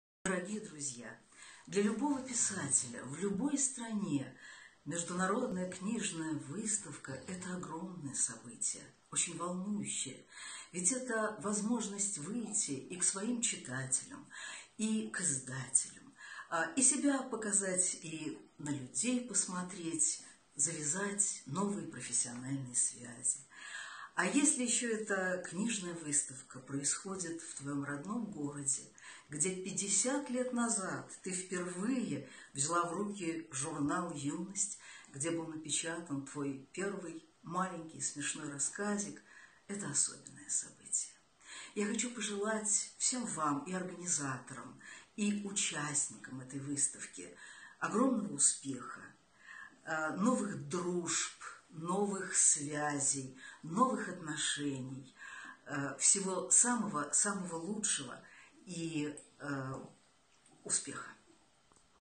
Исроилнинг русийзабон Ёзувчилар уюшмаси аъзоси Дина Рубина кўргазма иштирокчиларига видео мурожаати.
Ўзбекистон ССР Ёзувчилар уюшмаси (1978), СССР Ёзувчилар уюшмаси (1979), Халқаро ПЕН-клуб, Исроилнинг русийзабон Ёзувчилар уюшмаси (1990) аъзоси - Дина Рубинанинг кўргазма иштирокчиларига видео мурожаати.